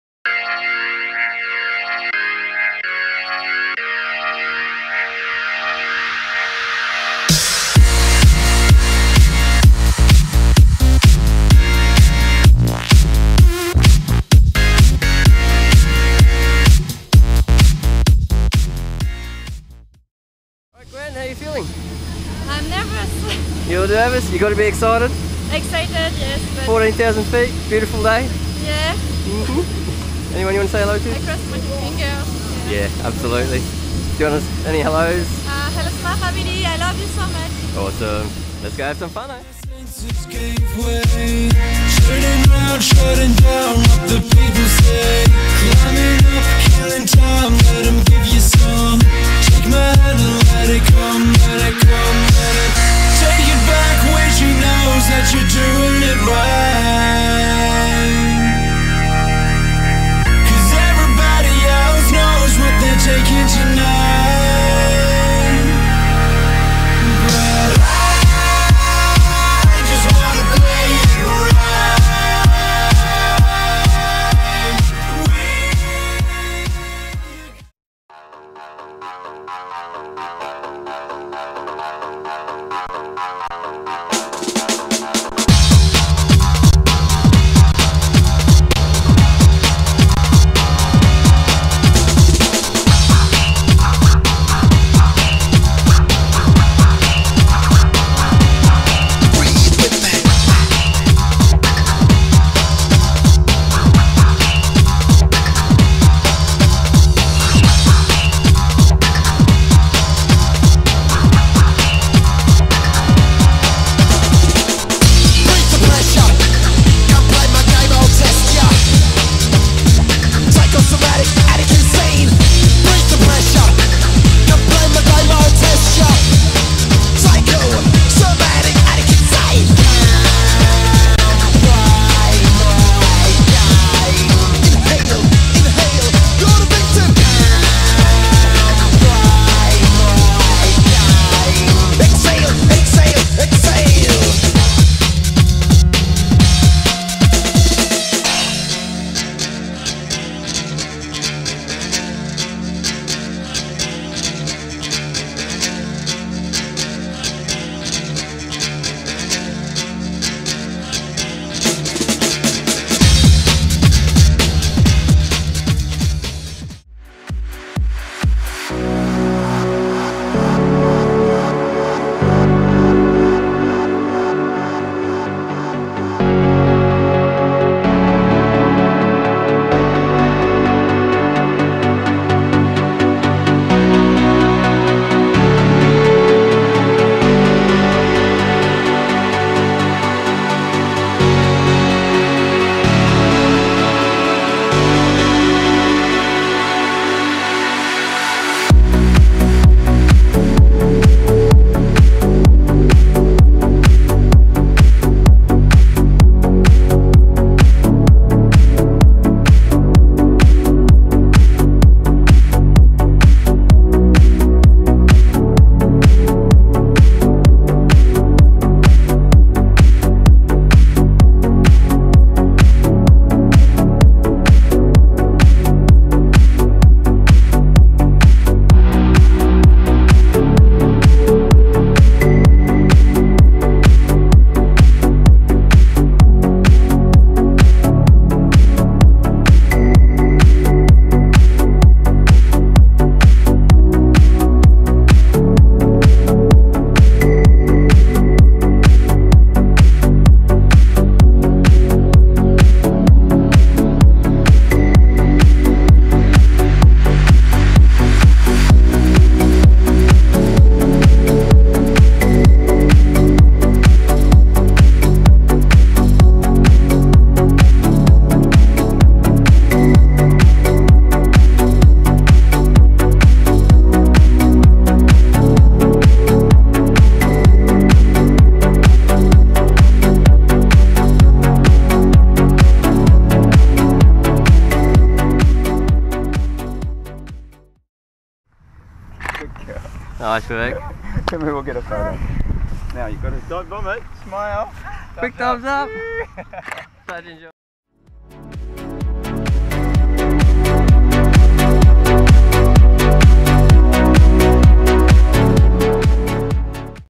Skydive of Gwendoline @Byron Bay
J'ai hésité, j'ai osé, je me suis précipitée, bref j'ai sauté !
Et j'ai ADORE :)